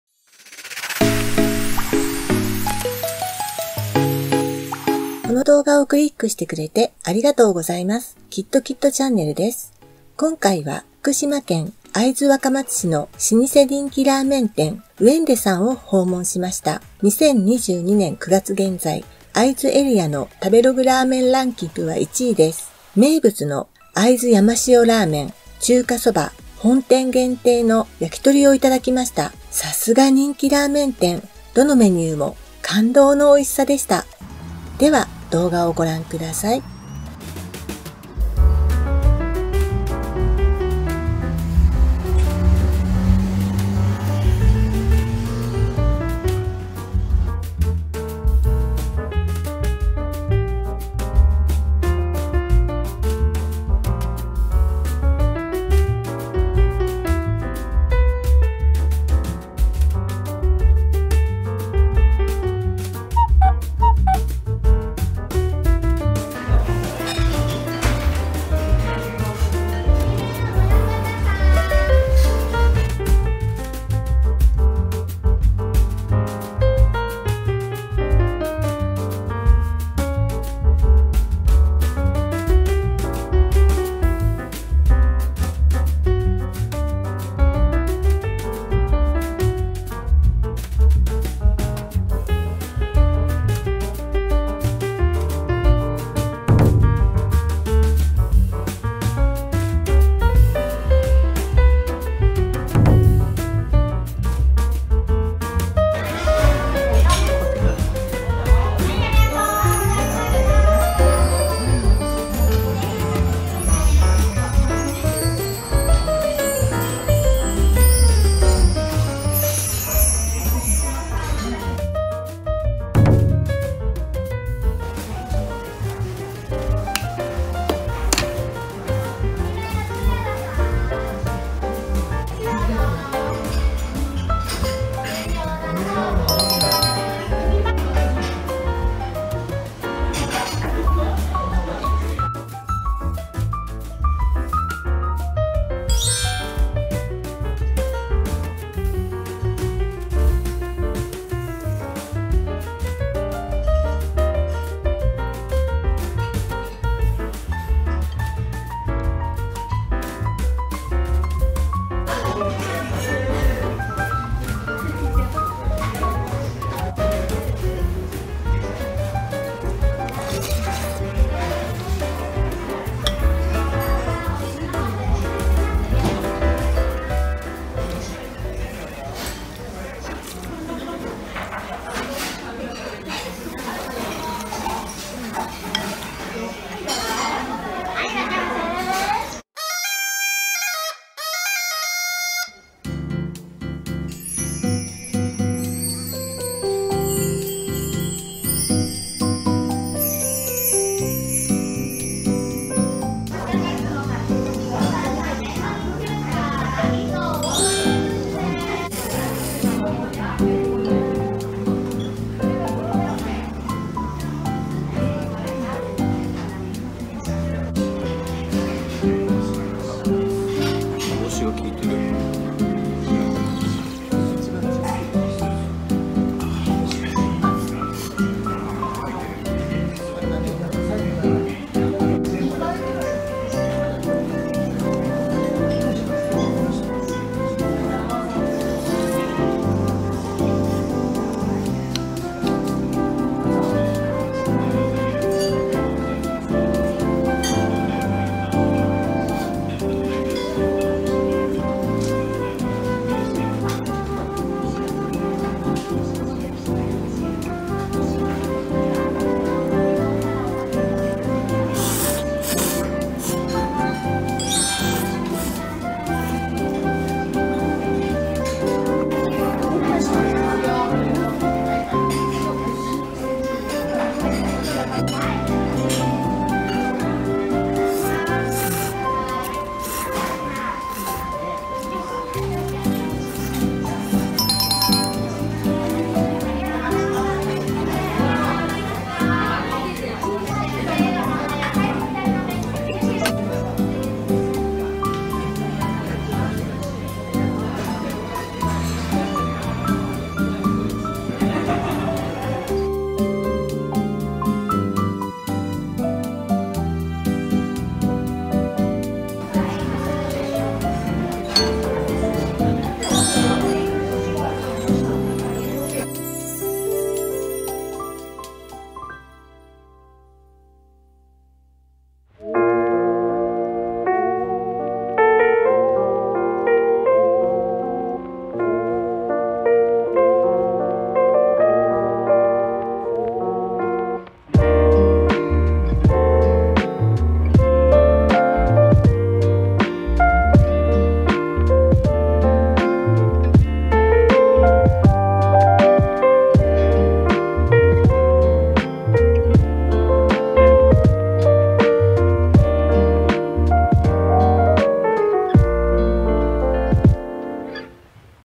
【激うまラーメン】「うえんで本店」絶品会津山塩ラーメンと絶対お薦め本店限定焼き鳥/福島県会津若松
➡︎高画質で視聴する場合は、画面右の設定（歯車⚙）から、高画質or1080ｐをクリック！➡︎

今回は、福島県会津若松市の老舗人気ラーメン店『うえんで』さんを訪問しました。
2022年9月現在の食べログランキングは1位です。
名物の山塩ラーメン、中華そば，本店限定の焼き鳥を頂きました。
さすが人気ラーメン店、どのメニューも感動の美味しさでした。